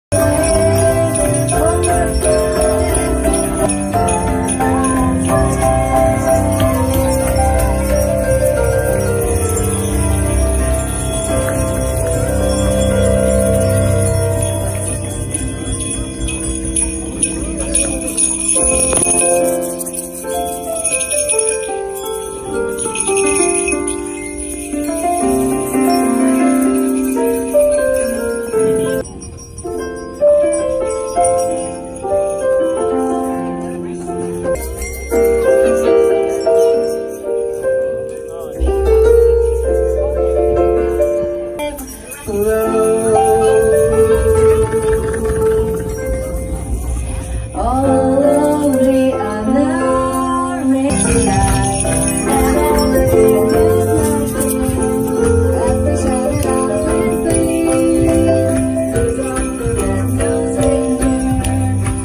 Christmas 2018 musical at Artground Goodman Arts Centre
A day out with the kids and TSM friends at Artground Arts Centre in December 2018